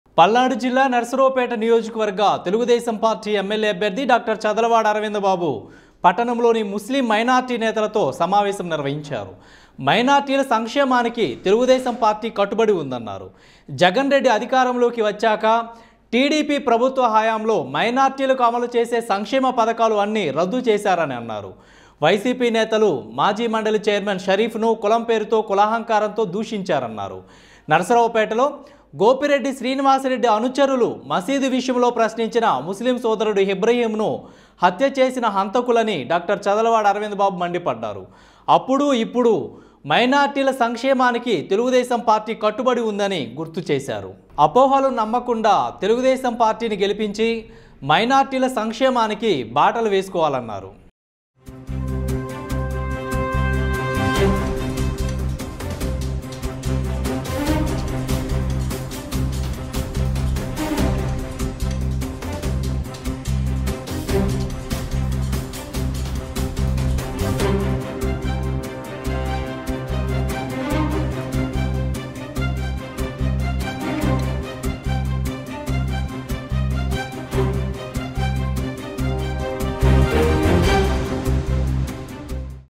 ముస్లిం సోదరలకు నరసరావుపేట టిడిపి అభ్యర్థి డా|| చదలవాడ భరోసా | NARASARAOPETA | PALNADU | CHADALAVADA
Bharat NO1 TV Official Channel